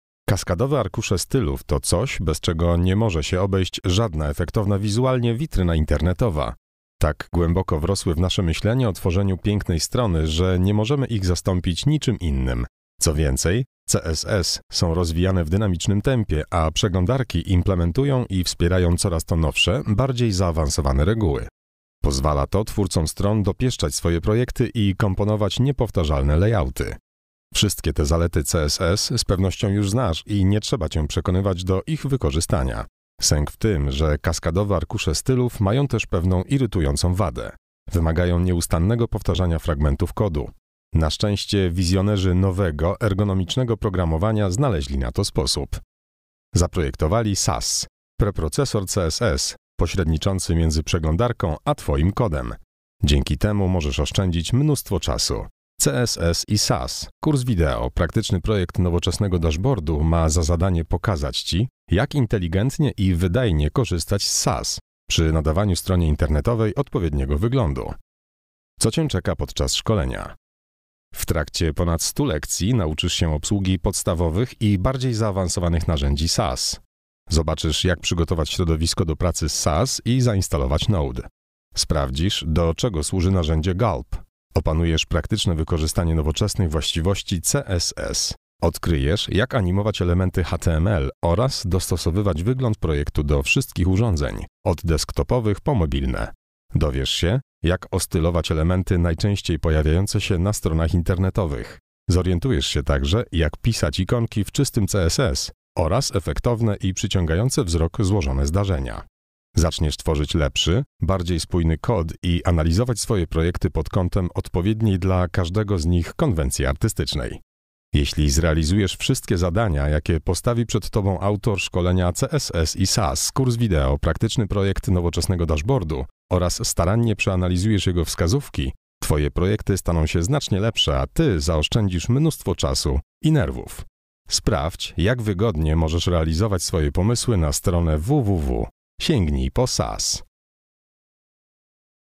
CSS i SASS. Praktyczny projekt nowoczesnego dashboardu [kurs video]
CSS i SASS. Kurs video. Praktyczny projekt nowoczesnego dashboardu ma za zadanie pokazać Ci, jak inteligentnie i wydajnie korzystać z SASS przy nadawaniu stronie internetowej odpowiedniego wyglądu. To niezwykle elastyczne środowisko udostępnia fantastyczne narzędzia i wzorce, które możesz wykorzystać w swoich projektach. Podczas kursu nauczysz się używać najbardziej efektownych i najwygodniejszych skrótów, trików i metod działania w środowisku SASS.
Sprawdź, jak wygodnie możesz realizować swoje pomysły na stronę WWW — sięgnij po SASS!

Żądny wiedzy?